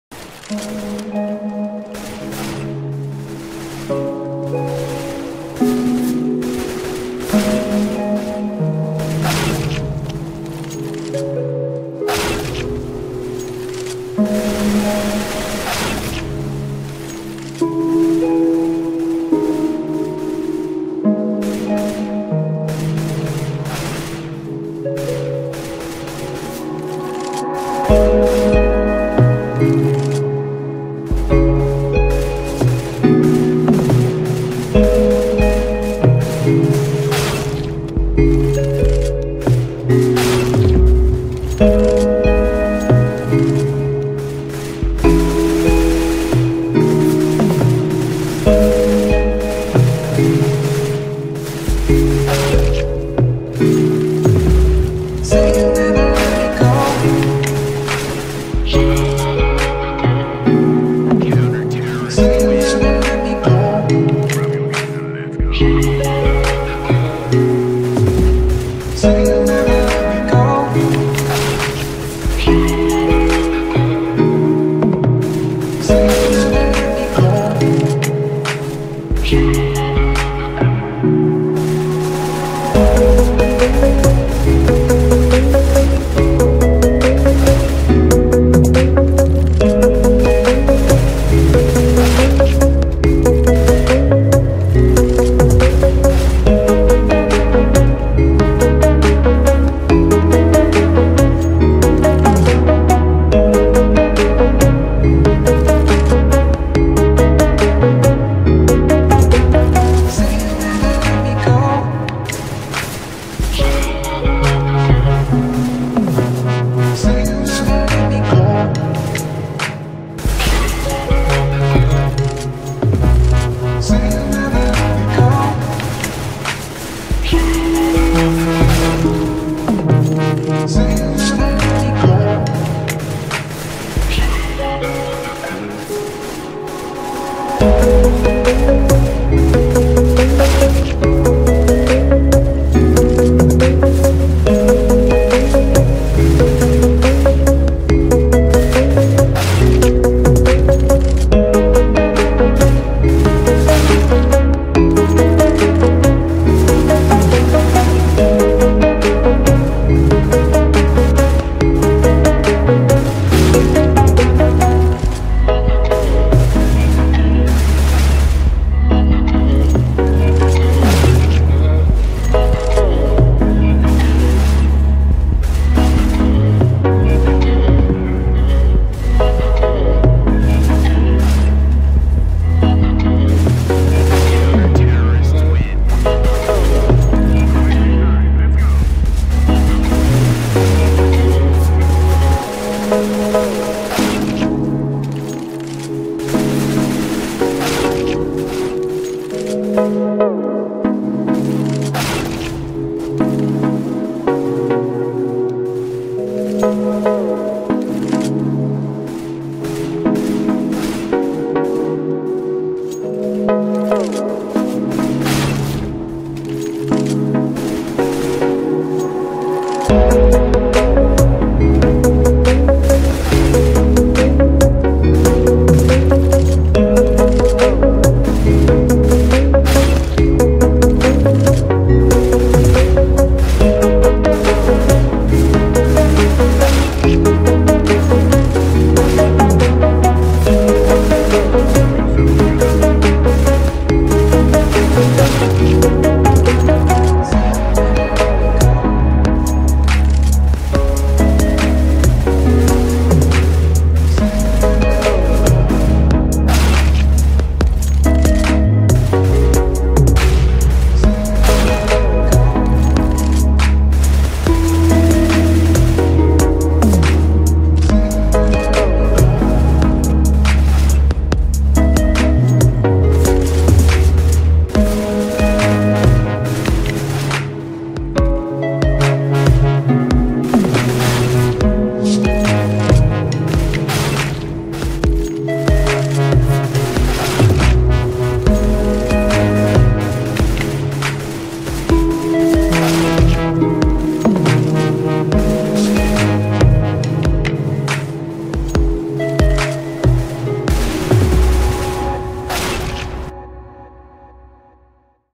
training on Titan HSMod server. (mediocre session :/ )
HS Only DM. Tried a new resolution, first time DMing in about a year because I wasn't so motivated to play seriously anymore so..
I also tried a new resolution (16:9) as shown on the video, with this crosshair aswell. Didn't do good at all but I guess it's worth showing for tikky to try to mimic those movements and add better aim than i had there ofc.